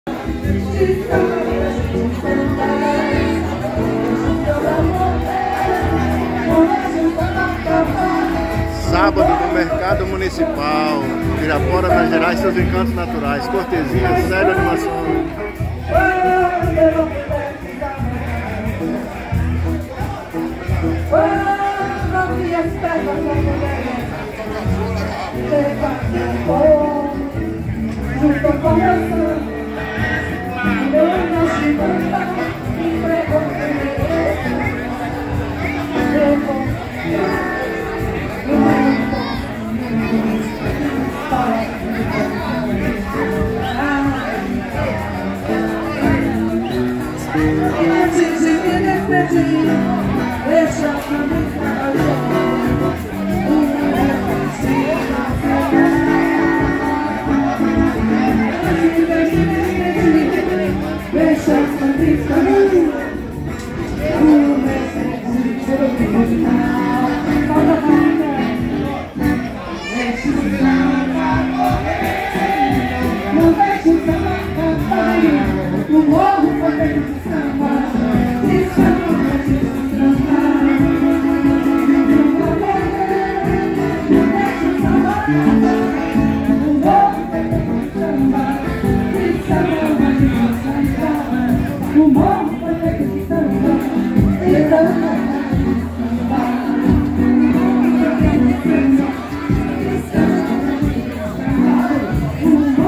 Sábado no mercado municipal.
Todo sábado, no mercado municipal,
Pirapora se enche de vida e melodia.
Na feirinha, o lazer é ritual,
música ao vivo em perfeita harmonia.
Os acordes suaves embalam o dia,
atraindo quem busca o simples prazer.
Risos, conversas, uma sinfonia,
um canto que faz o coração aquecer.
A feira, pulsando em cores e sabores,
é o palco de encontros, histórias e amores.
Todo sábado, em Pirapora, renasce
um momento de paz que jamais se esquece.
Jucélio Duarte Ferreira.
(Gerindo emoções).
Grato por visitar o meu perfil.
22/06/2024.